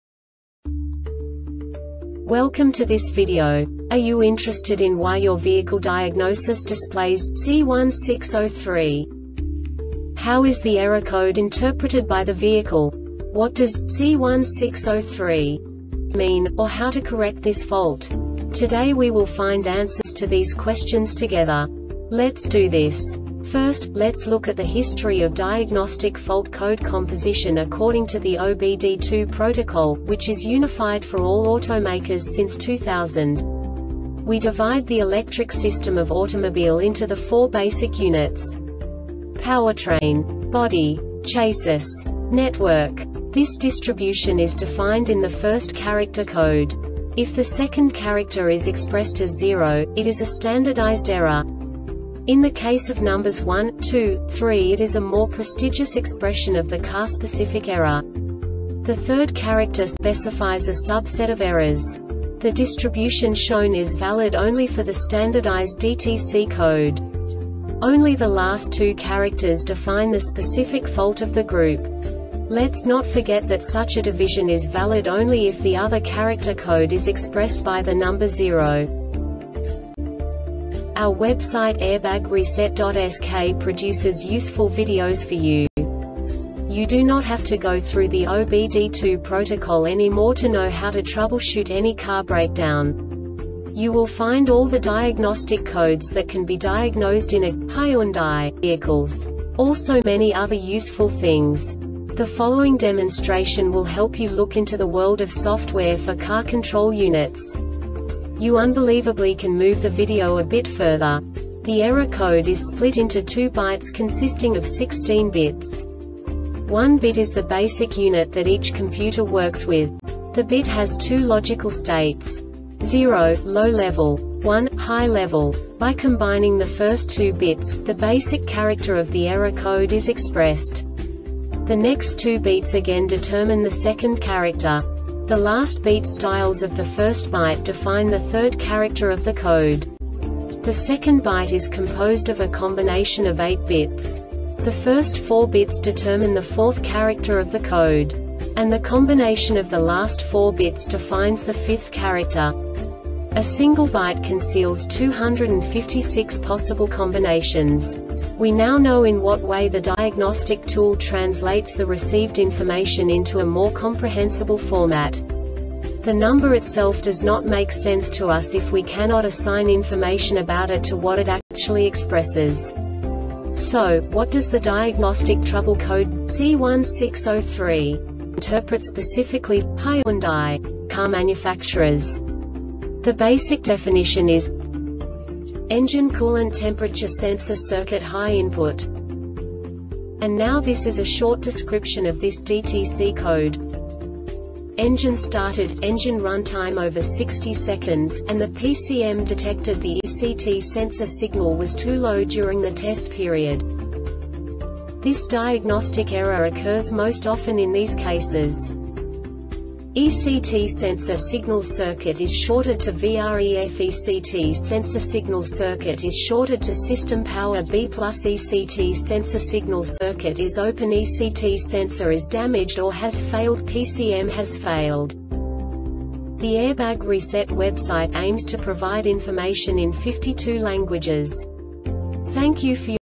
DTC Hyundai C1603 Short Explanation
Contents:
0:21 Basic DTC analysis according to OBD2 protocol standard.
1:48 Insight into programming
2:58 A diagnostically understandable description of the Diagnostic Trouble Code
3:05 Basic error definition

Make:
Hyundai
Code:
C1603
Definition:
Engine Coolant Temperature Sensor Circuit High Input
Description:
Engine started,engine runtime over 60 seconds,and the PCM detected the ECT sensor signal was too low during the test period.
Cause:
ECT sensor signal circuit is shorted to VREFECT sensor signal circuit is shorted to system power (B+)ECT sensor signal circuit is openECT sensor is damaged or has failedPCM has failed